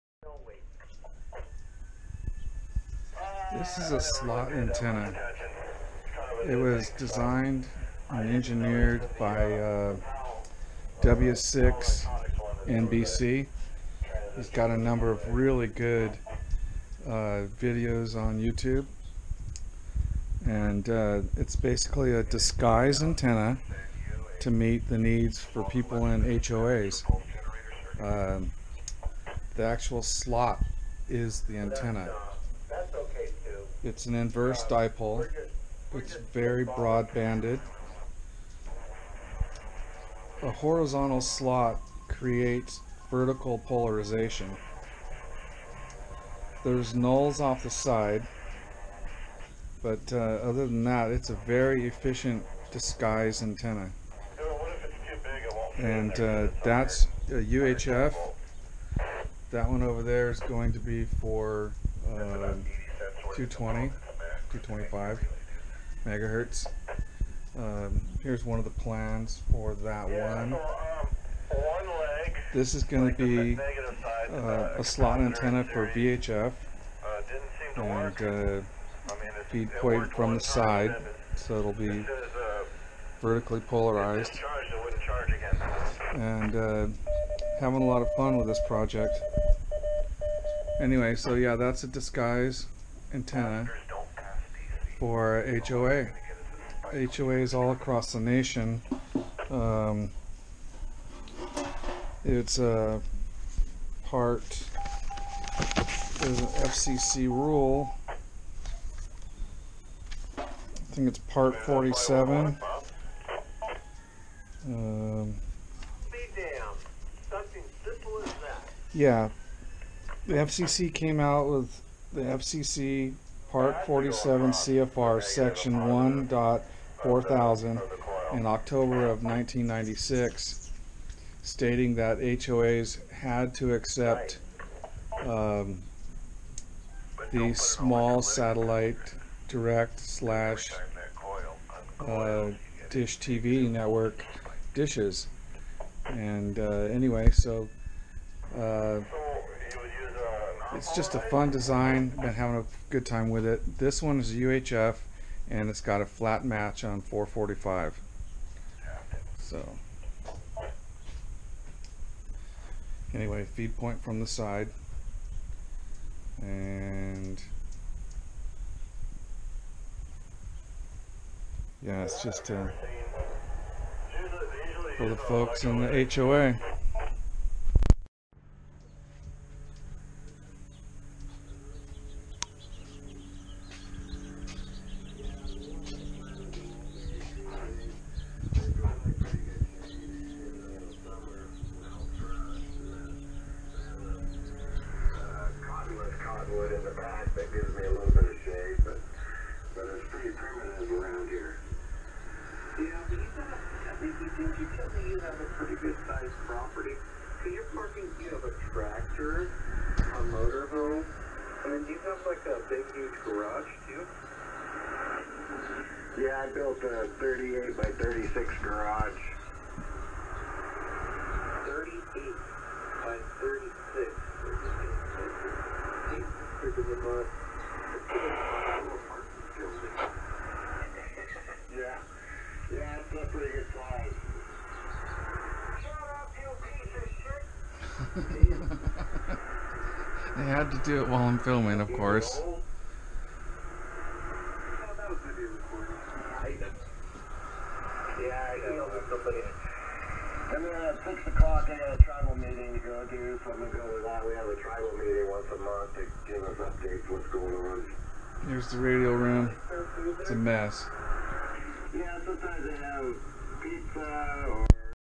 W6NBC Slot antenna, HOA approved, by KJ6KHV
W6NBC Slot antenna, a disquise HOA approved antennafor people trapped in HOA's or apartments. FCC 47CFR sec.1.4000 [Oct.1996] states mini dish antennas are legal in all HOA's nationally. This is a John Portune design.
                Enjoy KJ6KHV